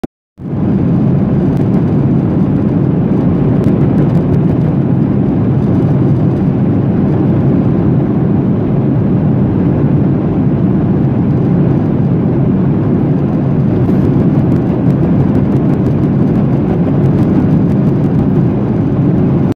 view of the wing at sunset
Moscow, Russia - August 30, 2017: Airplane of Rossiya airlines company in the air, view of the wing at sunset